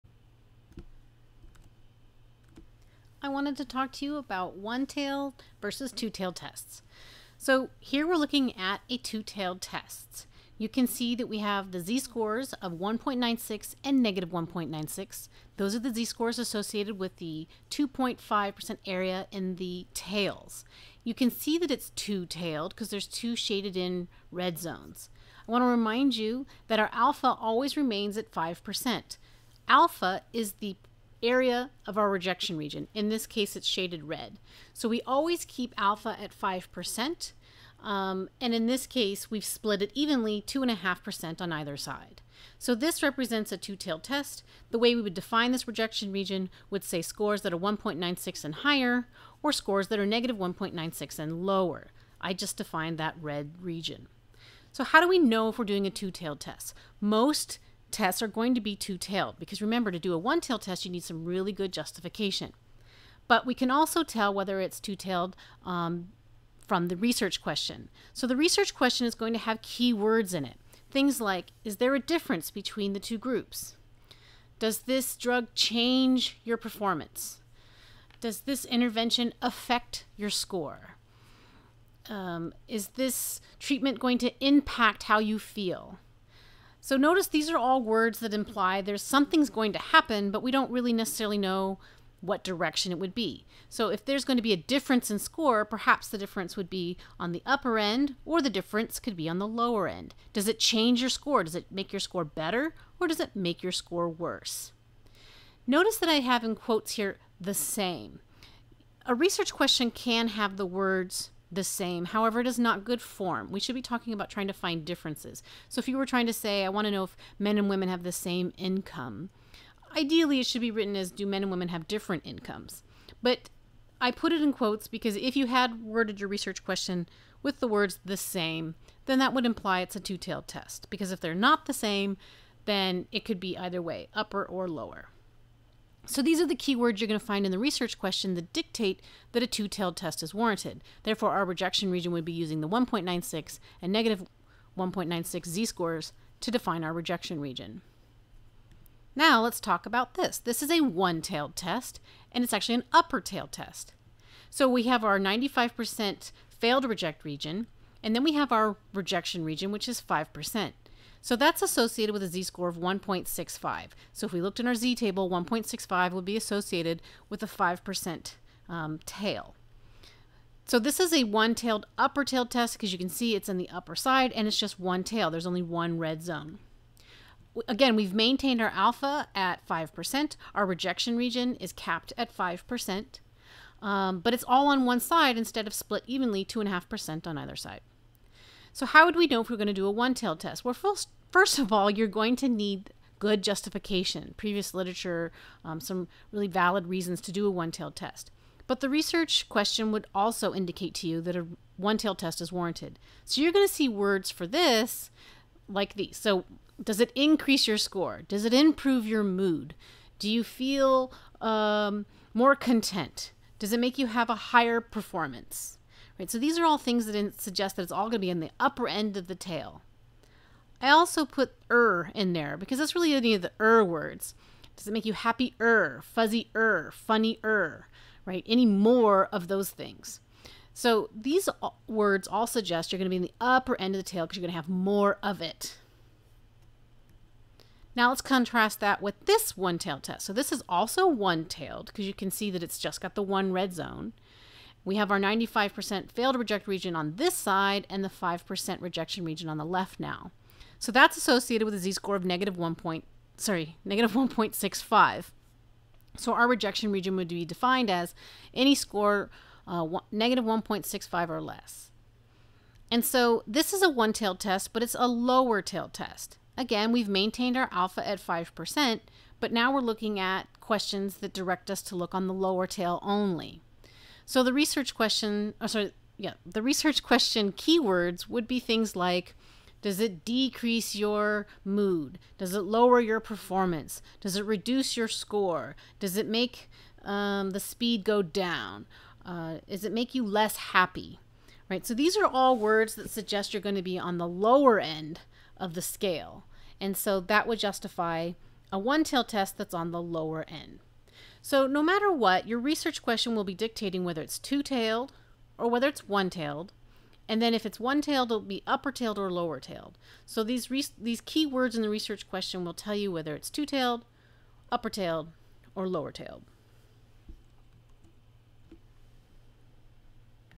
One tail vs Two tail tests
This video will help you determine whether you should be using a one tailed or two tailed test, based on the research question.